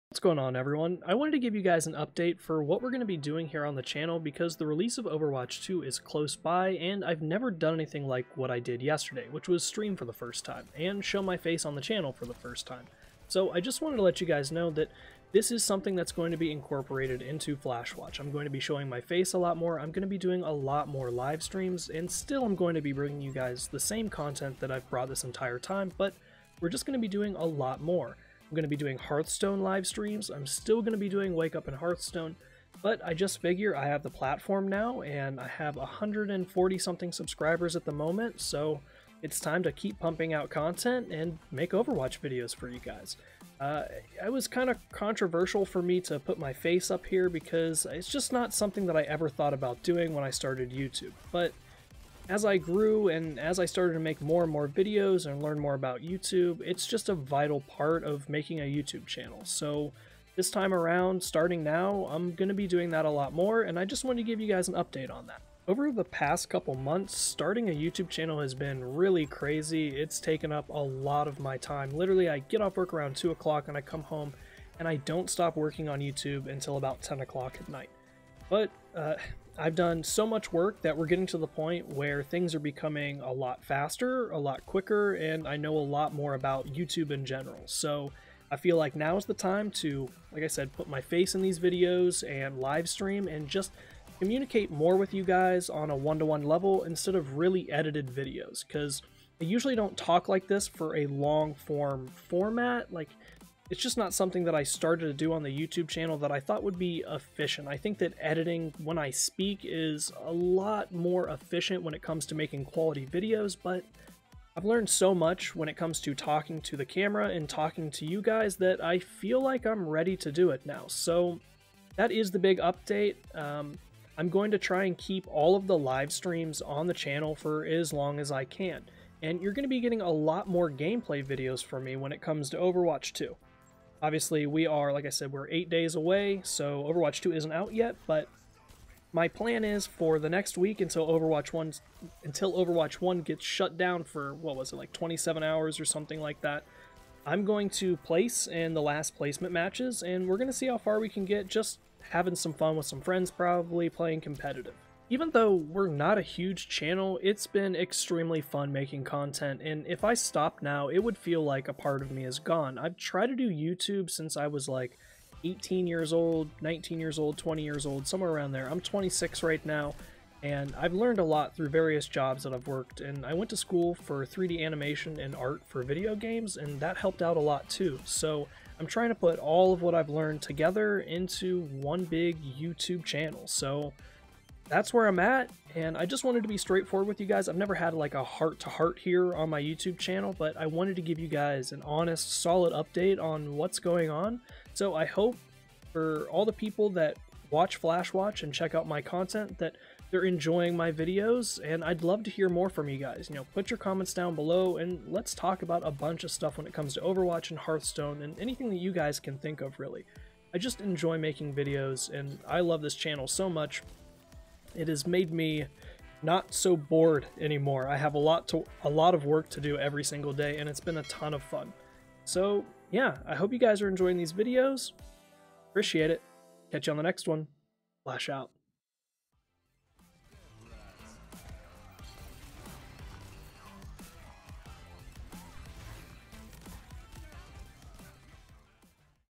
Big Plans For The Future Of The Channel: (OVERWATCH 2, HEARTHSTONE)
This video is a big channel update. Were starting to do a lot more here on the channel so I wanted to keep you guys up to date! I hope you've been enjoying my content and enjoy this video! Thanks everyone!